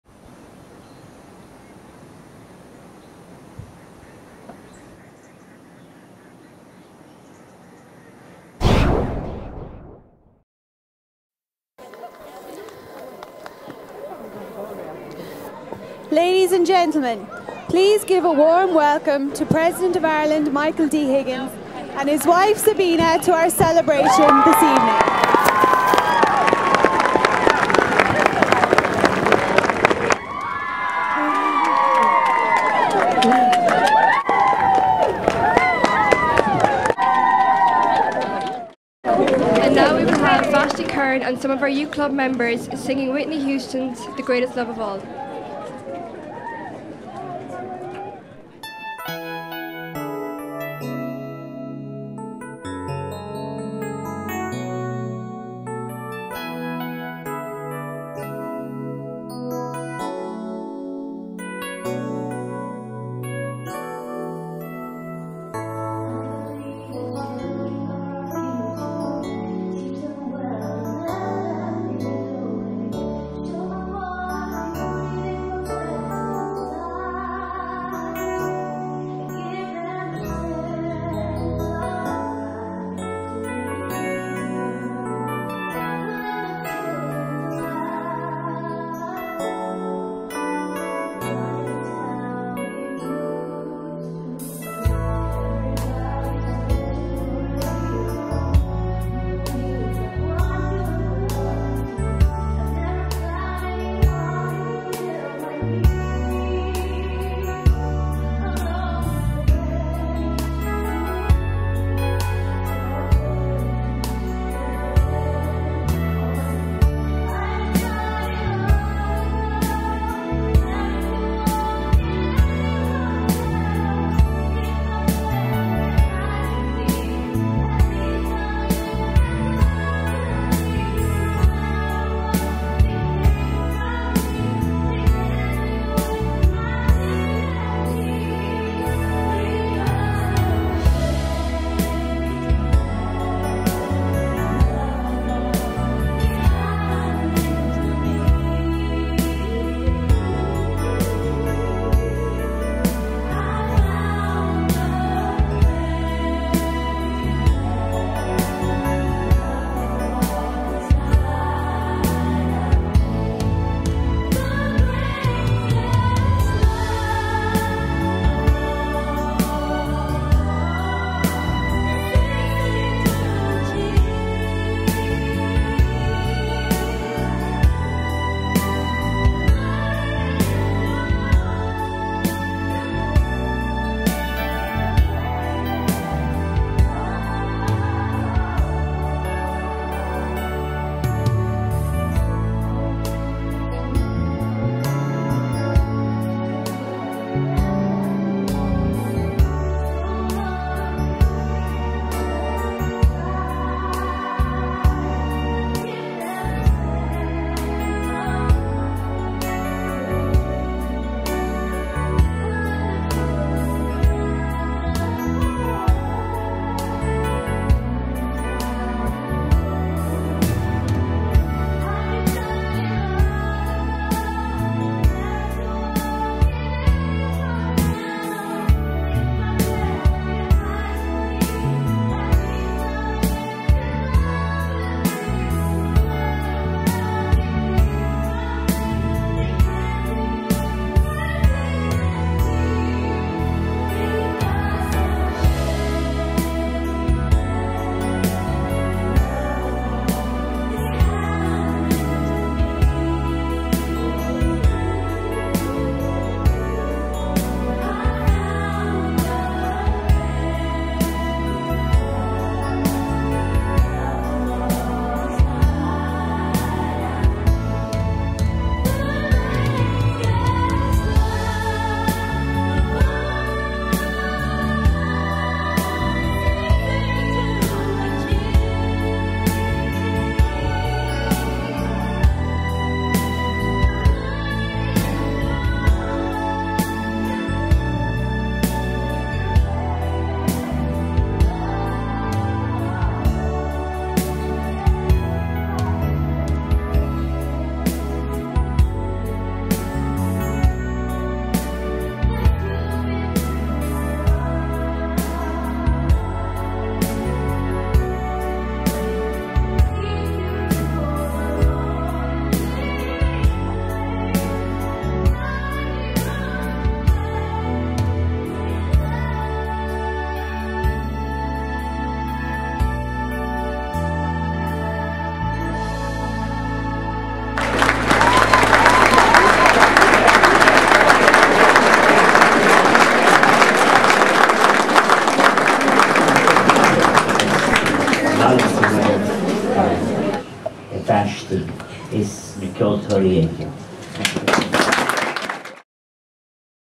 Vashti Curran sings for the president of Ireland
Rising star Vashti Curran sings for the President of Ireland.  You could hear  a pin drop as this young woman with a remarkable voice gave a magical  performance.  Vashti is currently based in London, where she is persuing her singing and acting career.